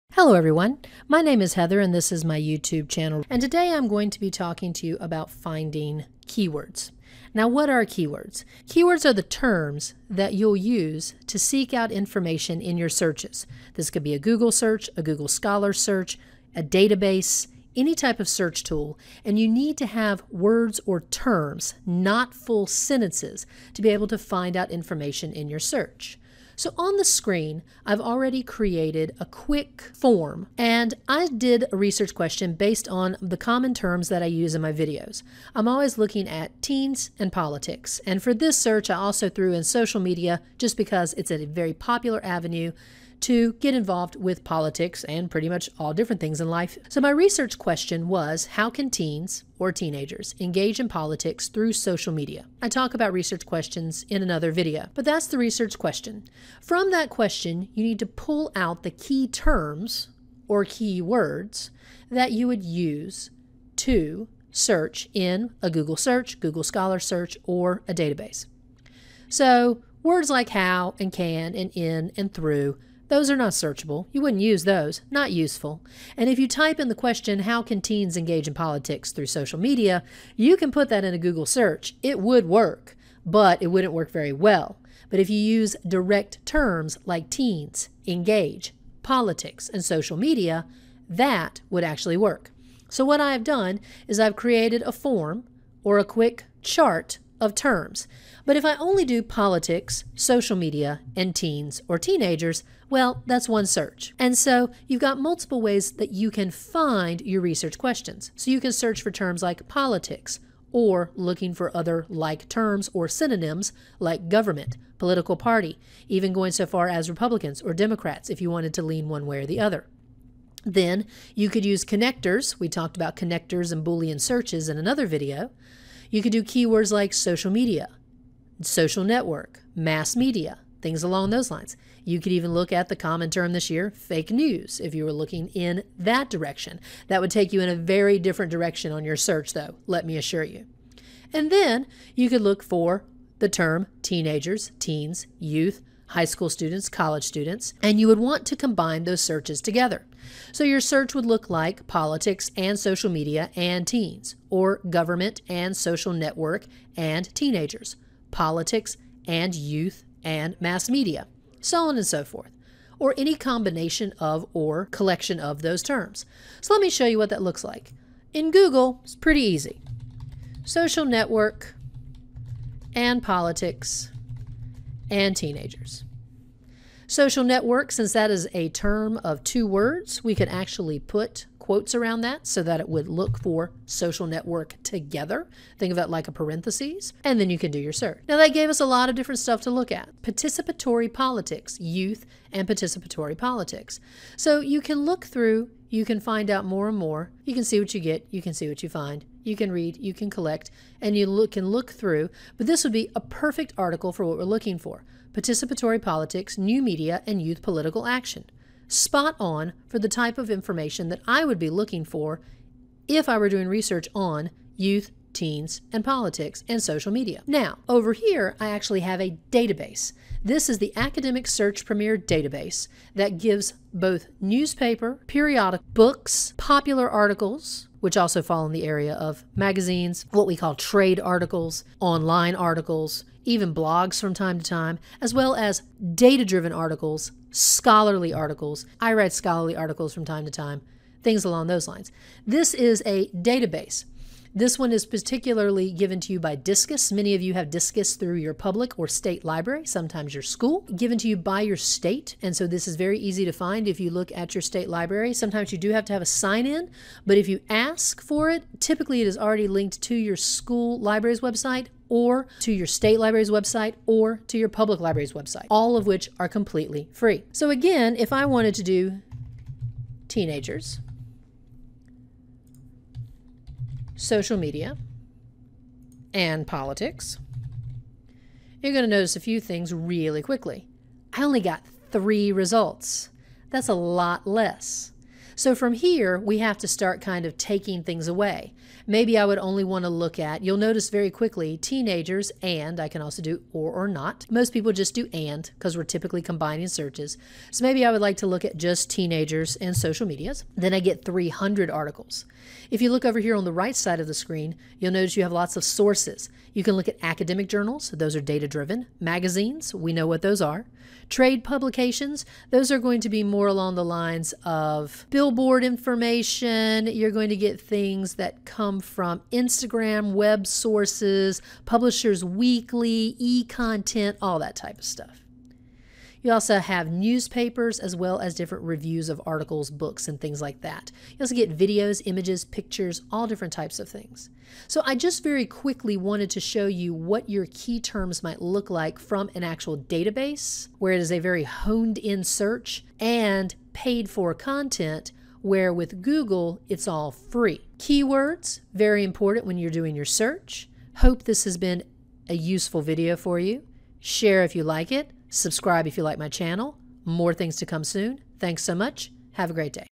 Research Keywords
Figure our keywords for your next search in this short video.